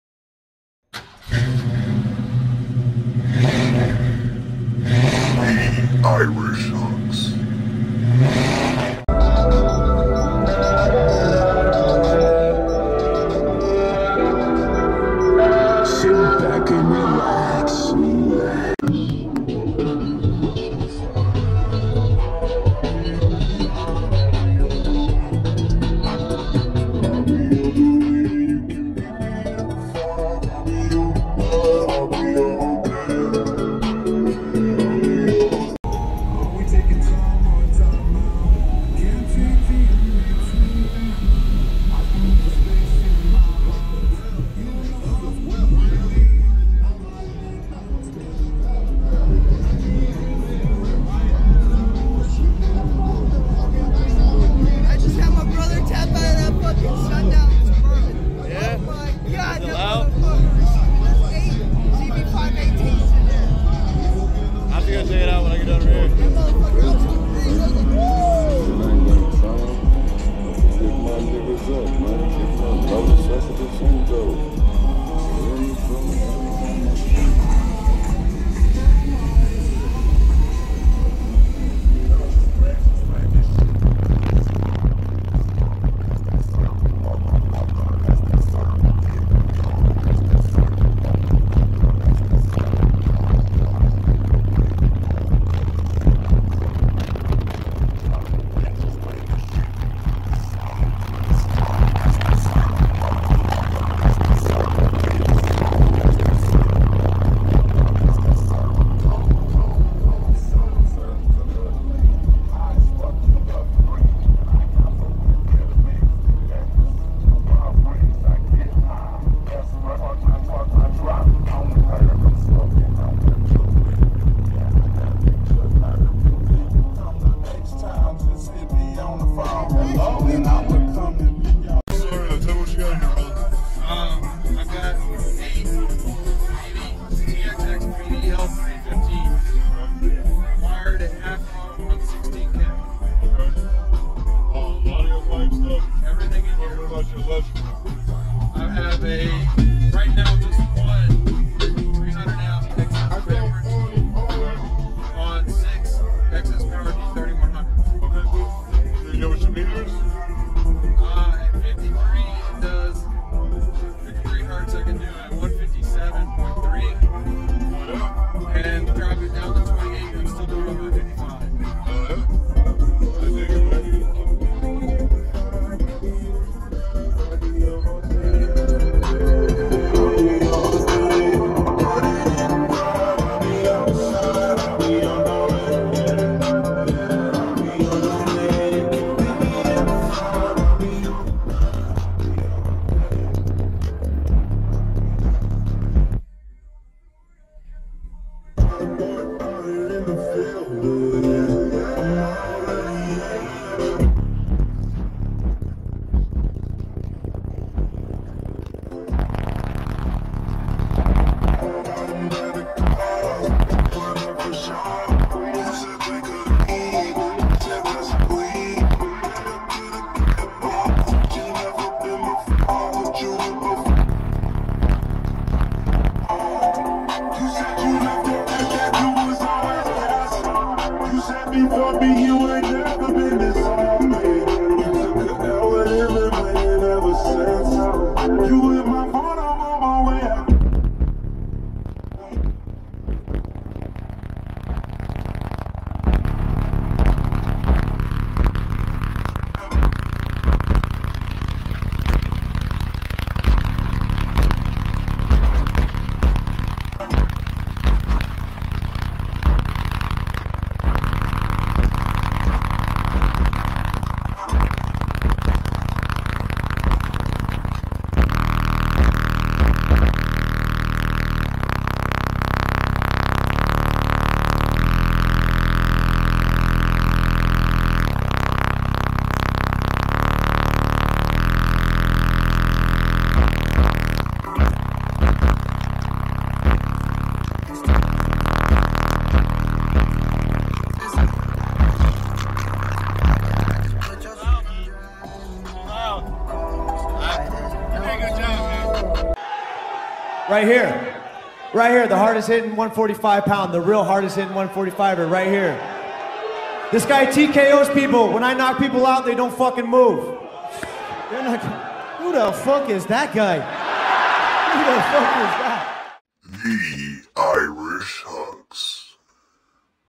8 Audiopipe TXX BDL3 15's on 16k | All Audiopipe Build
Welcome back bass heads!

In this video we have 8 Audiopipe TXX BD 15's on 16k of Audiopipe power! This footage is from the TDH show 2019. Be sure to hit the notifications buttons so you don't miss out on new uploads!